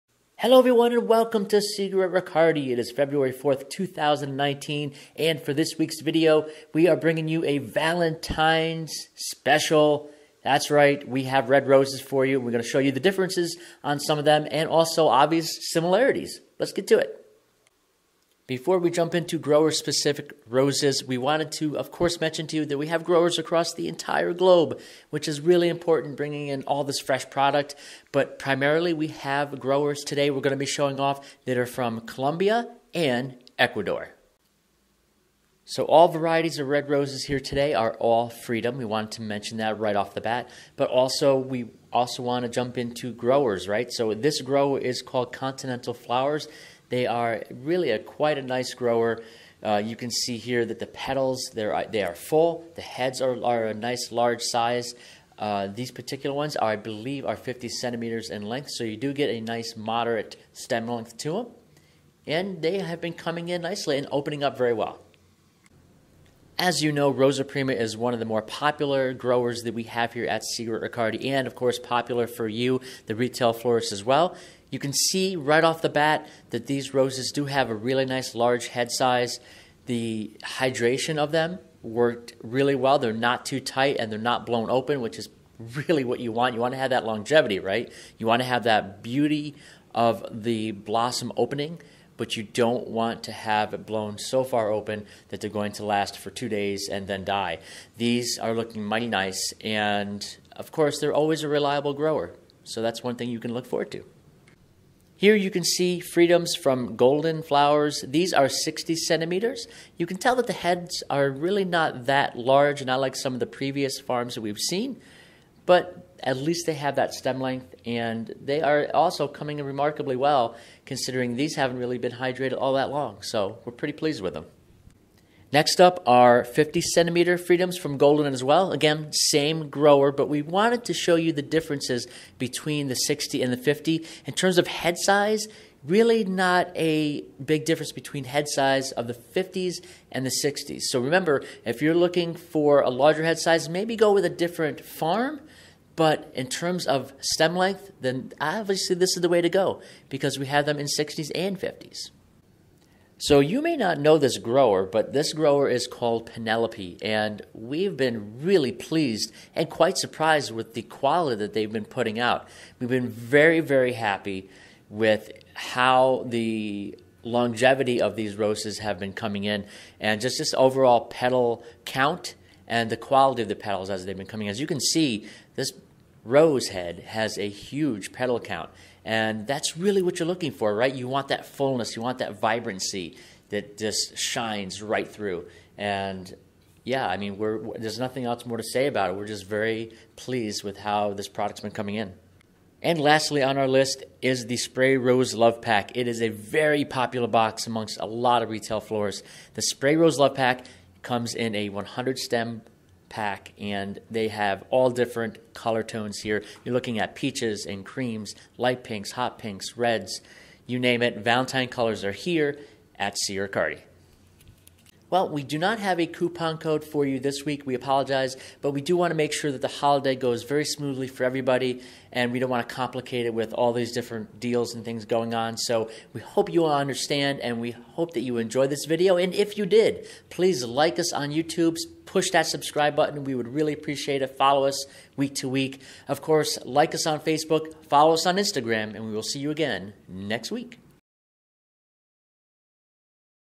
Seagroatt Riccardi's Valentine Red Roses 2019
This week we are showcasing a Variety of red roses called Freedom's and a select group of growers that grow them!!